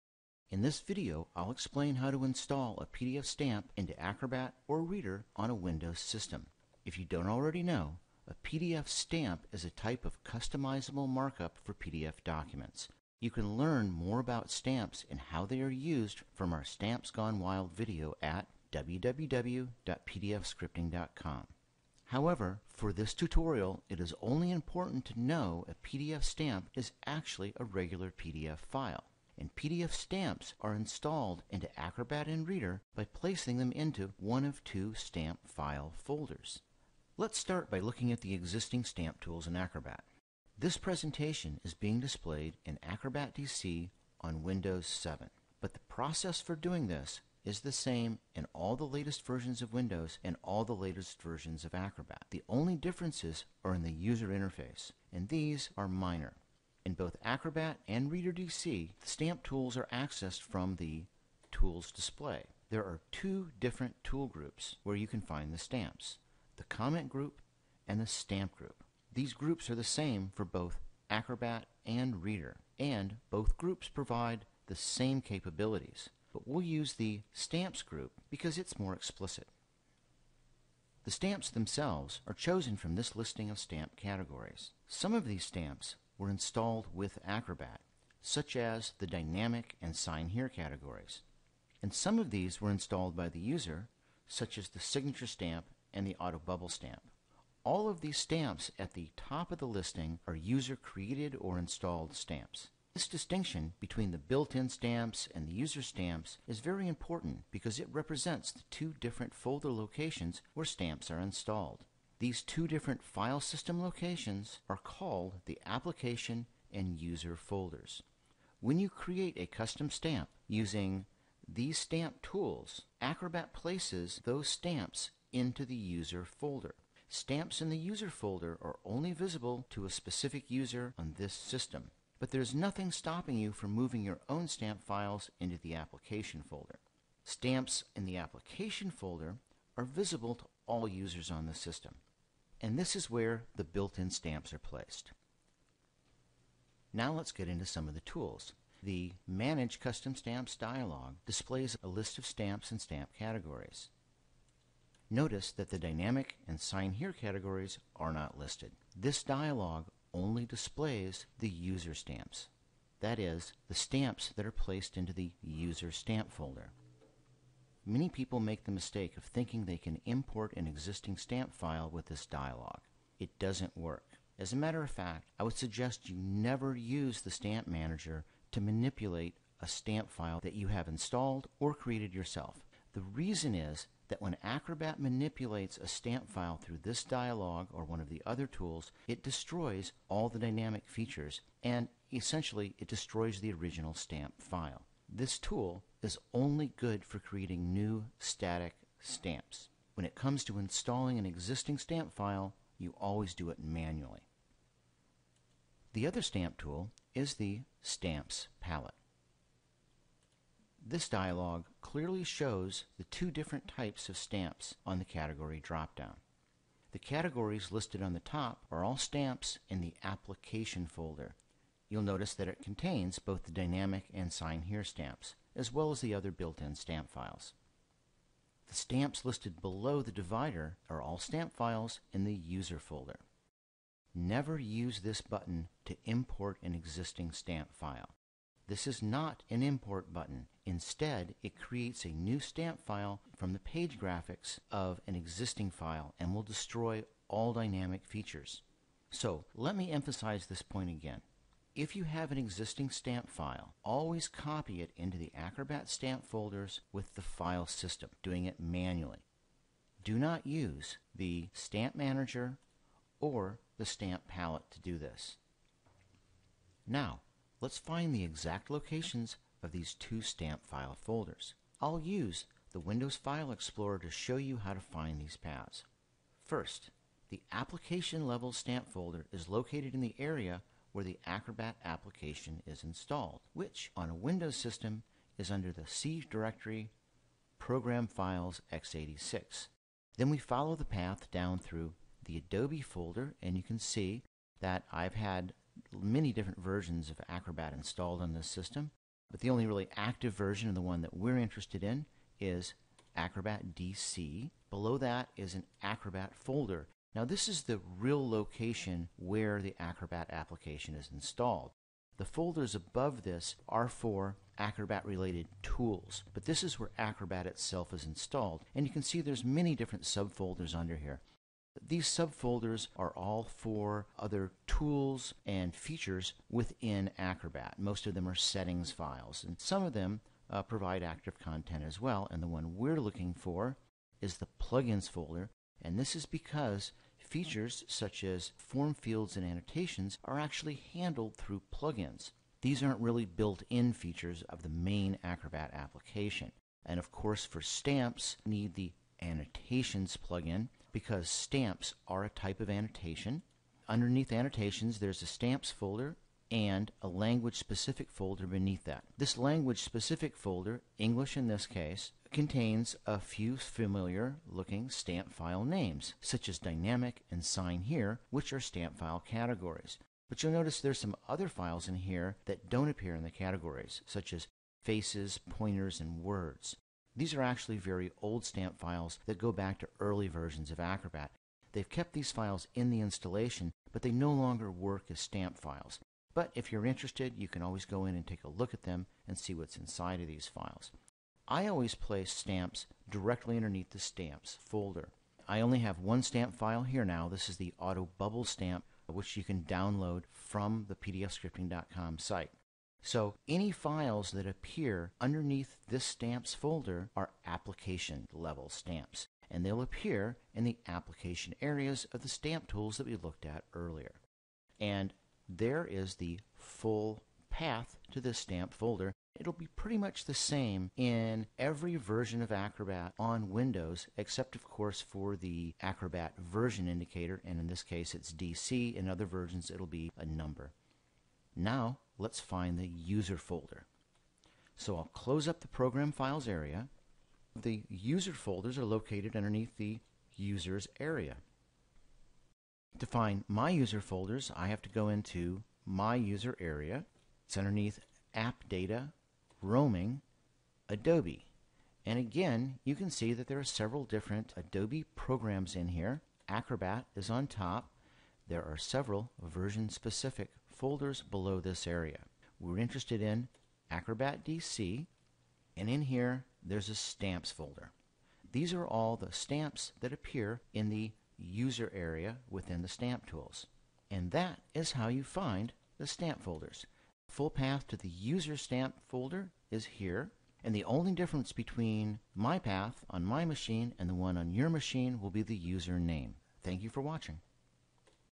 Installing a PDF Stamp Into Acrobat DC on Windows
This tutorial explains in detail how to locate the two folders on an MS Windows system where stamps are installed in Acrobat. Although it is shown in Acrobat DC, this same technique applies to any version of Acrobat.
It Also explains why there are two folders, and how you can tell from the Acrobat UI in which folder a particular stamp is installed.